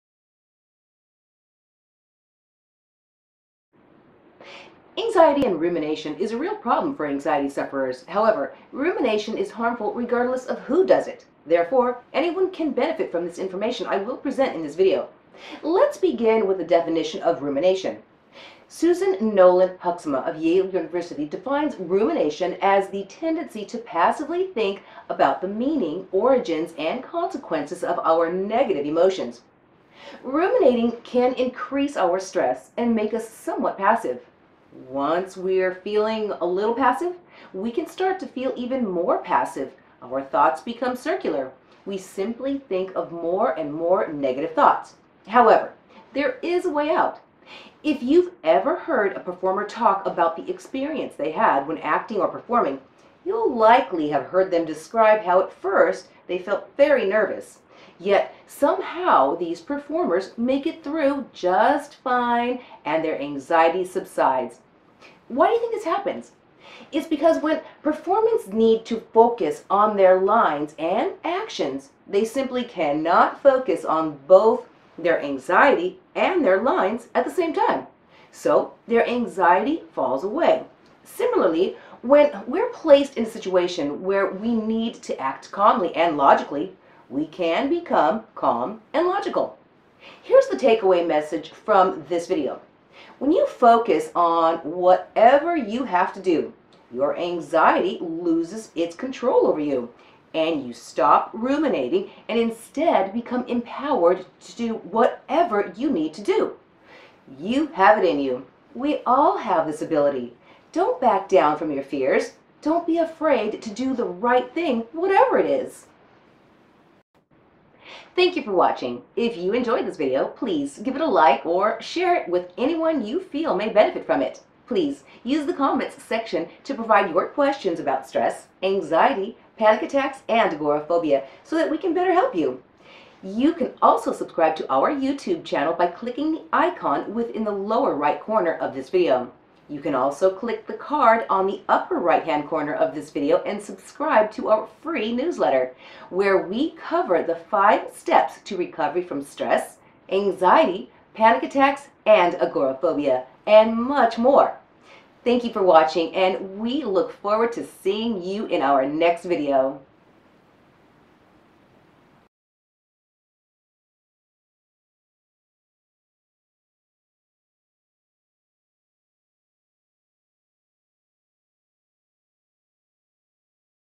ADHD and Anxiety Tips: How to Stop Rumination Now
Rumination is a real problem for anxiety sufferers. It's harmful regardless of who does it.  Therefore, anyone can benefit from the information we will present in this video on how to stop rumination.

Let's begin with a definition of rumination.  Susan Nolen-Hoeksema of Yale University defines rumination as the tendency to passively think about the meaning, origins, and consequences of our negative emotions.

Ruminating can increase our stress and make us somewhat passive.  Once we're feeling a little passive, we can start to feel even more passive. Our thoughts become circular. We simply think of more and more negative thoughts.

However, there is a way out.

If you've ever heard a performer talk about the experience they had when acting or performing, you'll likely have heard them describe how at first they felt very nervous. Yet somehow these performers make it through just fine and their anxiety subsides.

Why do you think this happens?

It's because when performers need to focus on their lines and actions they simply cannot focus on both their anxiety and their lines at the same time, so their anxiety falls away.

Similarly, when we are placed in a situation where we need to act calmly and logically, we can become calm and logical.

Here is the take-away message from this video:  When you focus on whatever you have to do, your anxiety loses its control over you, and you stop ruminating and instead become empowered to do whatever you need to do.  You have it in you.  We all have this ability.  Don't back down from your fears.  Don't be afraid to do the right thing, whatever it is.